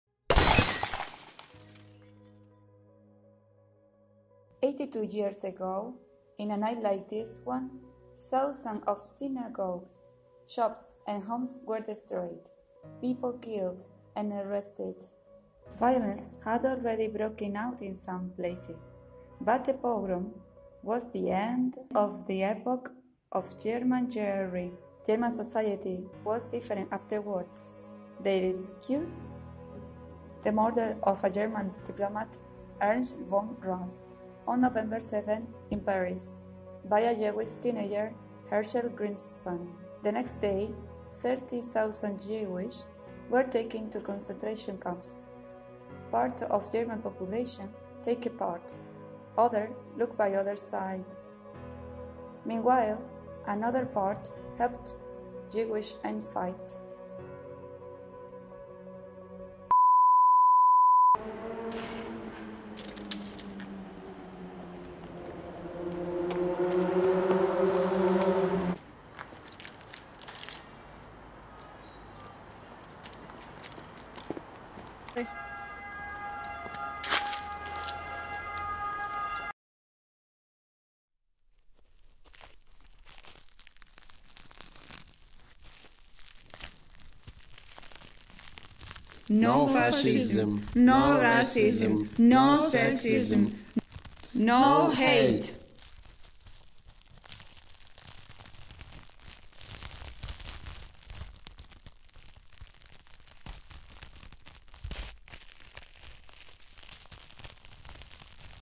International day against fascism and antisemitism 9th november
Fascism brings to our lives hate and violence and it is necessary to remember it for not forgetting. Why the 9th of November is the International Day Against Fascism and Antisemitism? What has this date that is important to remember? Check the video to get to know more and share if you like it.
This video was made by Lidia and Leticia during their volunteering in Palermo in the frame of the project ‘’Empowerment through volunteering activities’’ funded by Erasmus+.